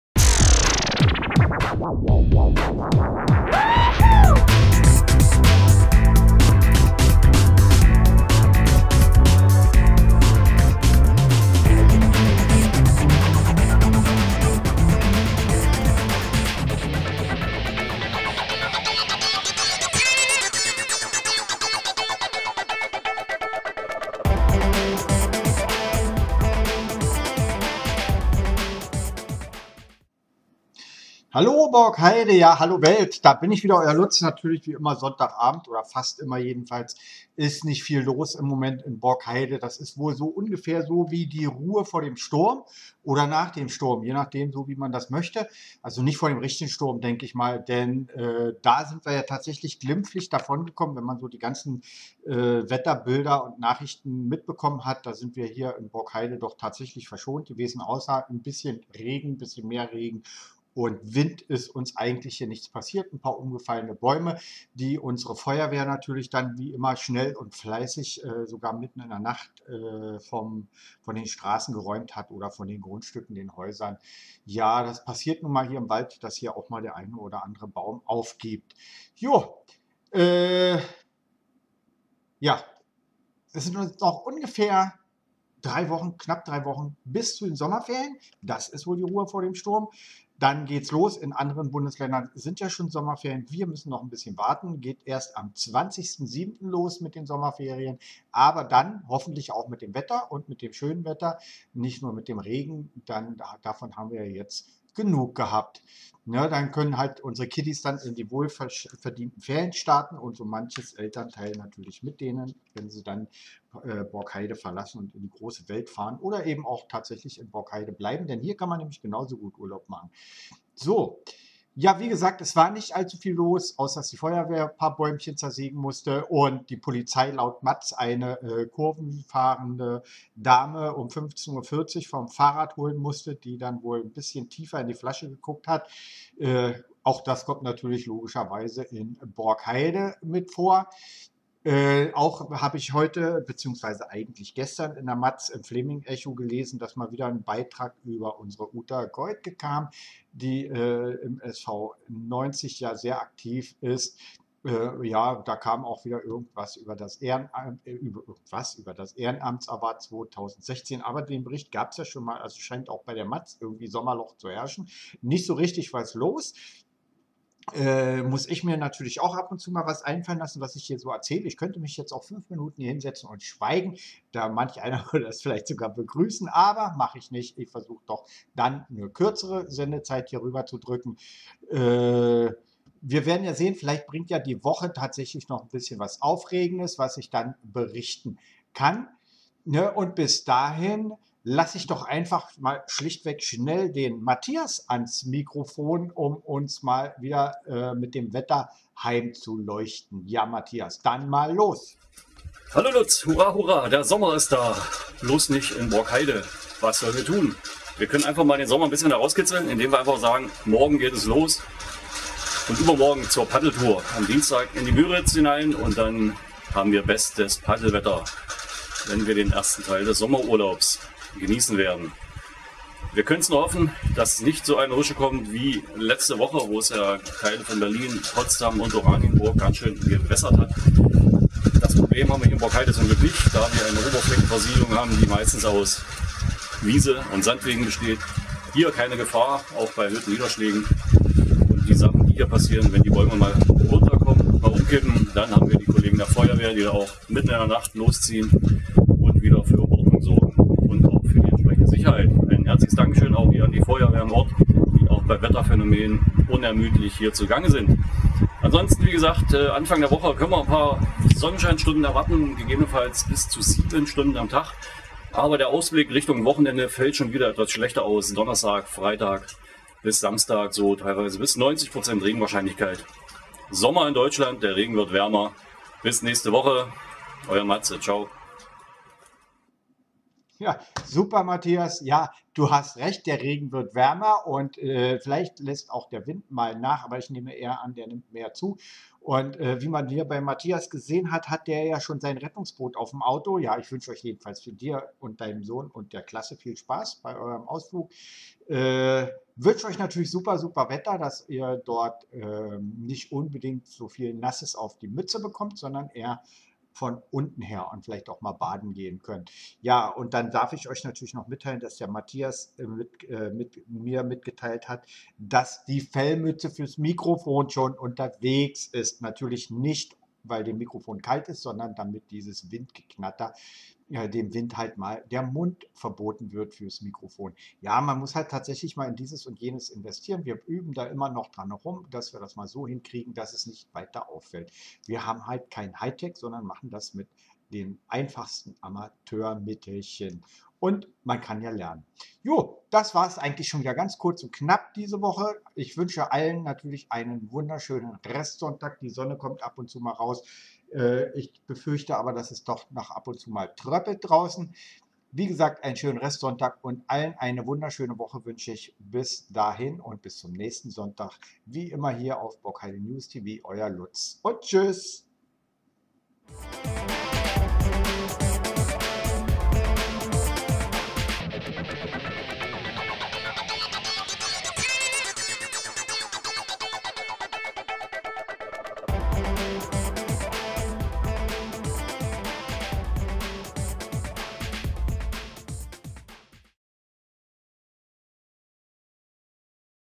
Borkheide News TV
Information der Woche aus Borkheide; schwankende Radler; Wind und Wetter; Sommerloch oder was?